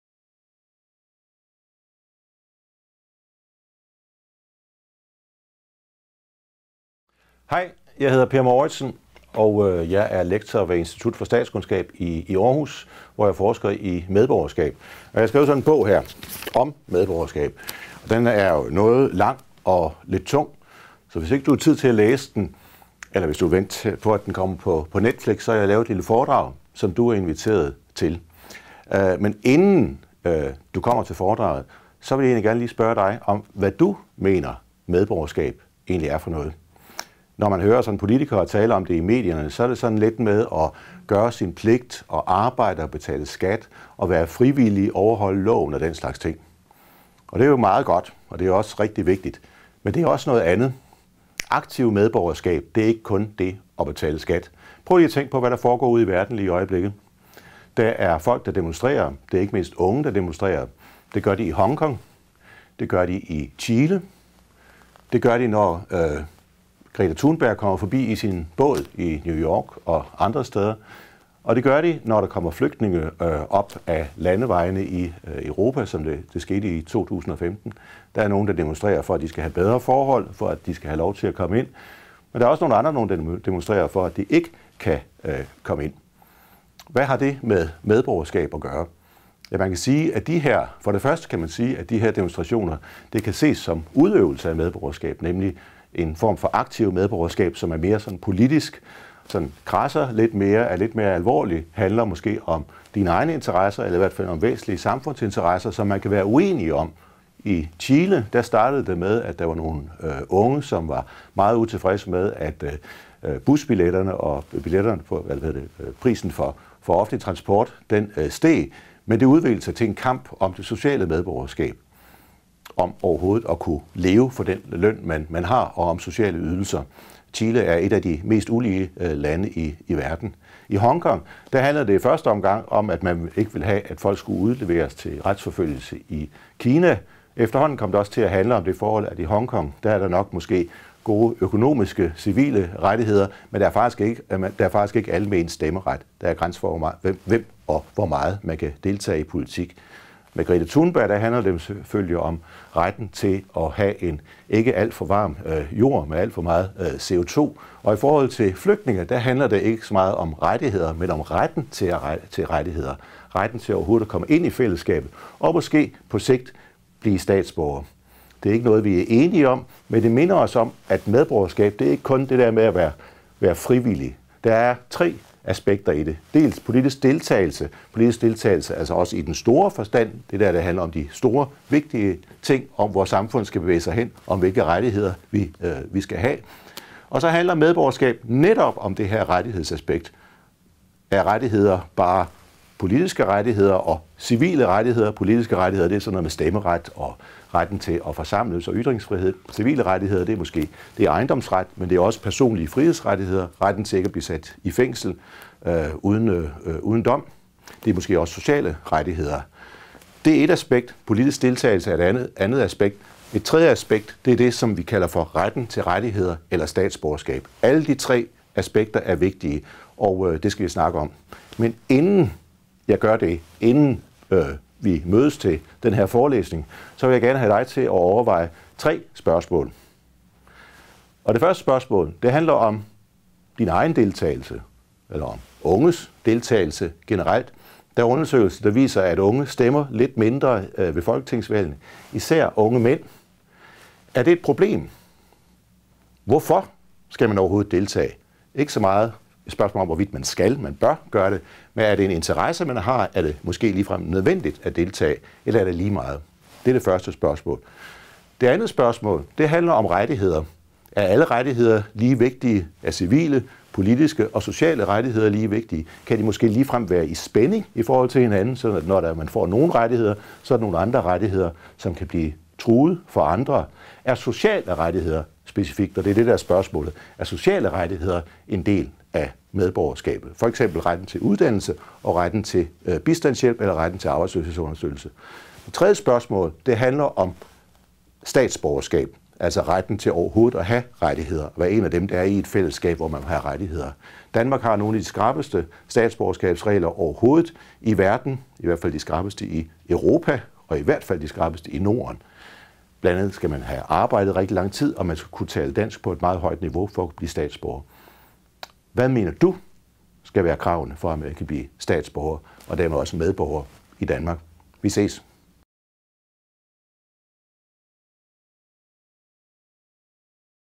Per Mouritsen: Medborgerskab: Rettigheder, deltagelse og medlemsskab
Kom og hør - eller se via streaming - Lektor Per Mouritsen, Institut for Statskundskab, Aarhus Universitet, forelæse over emnet Medborgerskab i Danmark: Rettigheder, deltagelse og medlemskab.

Medborgerskab er politisk hurra-ord nummer et - i kommunerne, på udlændingeområdet, i skoler og gymnasier. At være medborger er at gøre noget godt for fællesskabet, deltage i politik, gøre sin pligt – og have det man i gamle dage kaldte ”borgerdyd”. Men i Danmark er begrebets bredere betydning  blevet fortrængt.